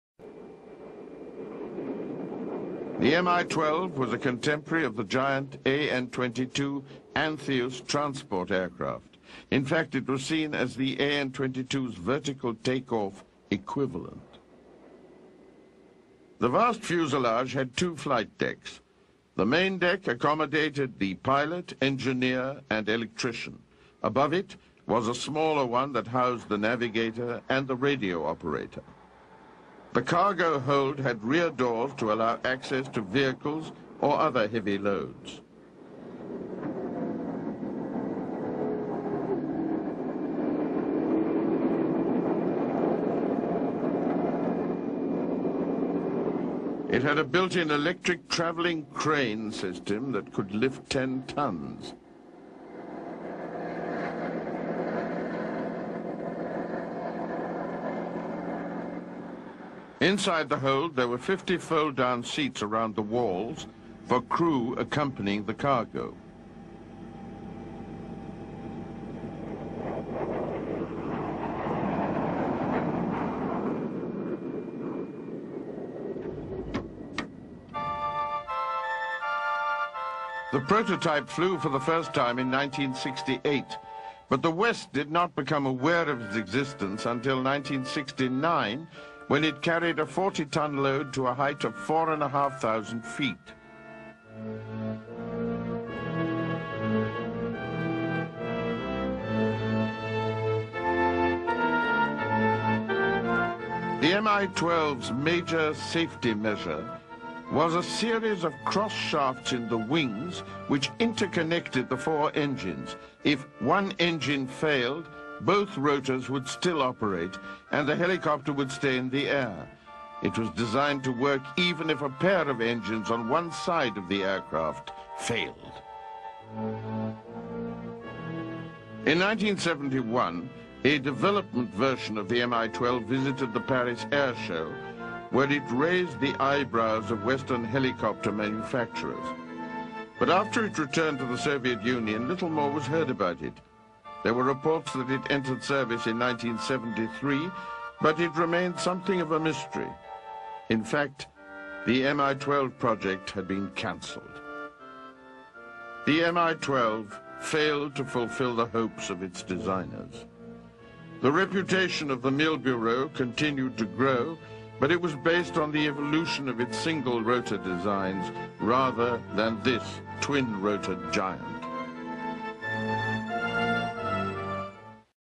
Maior helicóptero do mundo já construído - Mil Mi 12 - V12
O Mil Mi-12, também conhecido como V-12 (apelido NATO Homer) era simplesmente gigantesco.
Em agosto de 1969, esse mega-helicóptero conseguiu erguer uma carga de mais de 54 toneladas a uma altura de 2.255 metros, um recorde mundial que permanece até hoje.